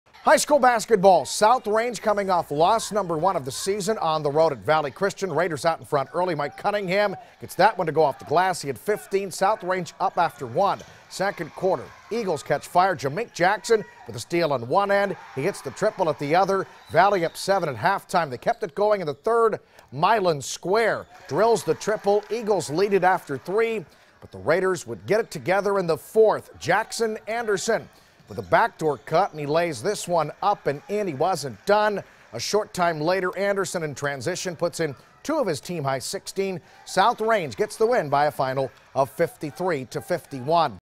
South Range rallies to top Valley Christian
South Range topped Valley Christian 53-51 Tuesday night in boys' high school basketball action.